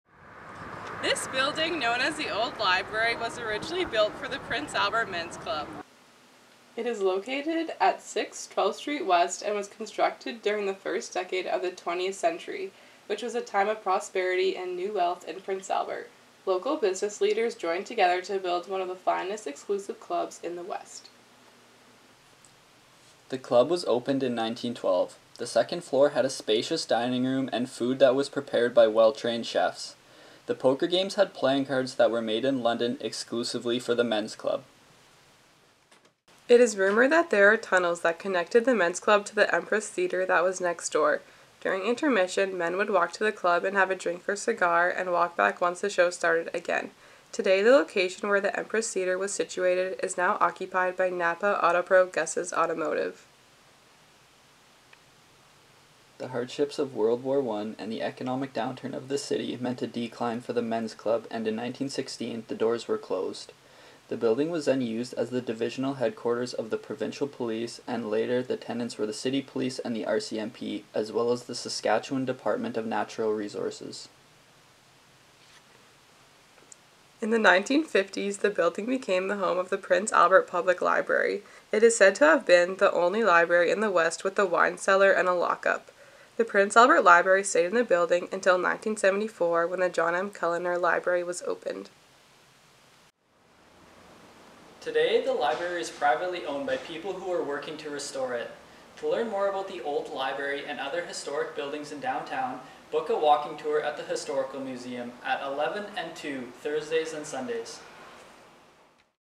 Prince Albert Historic Buildings - The Old Library
Do you remember visiting the Old Library in downtown Prince Albert? Learn about the fascinating history of the building and it's many purposes in this video from Zac & Mikaela!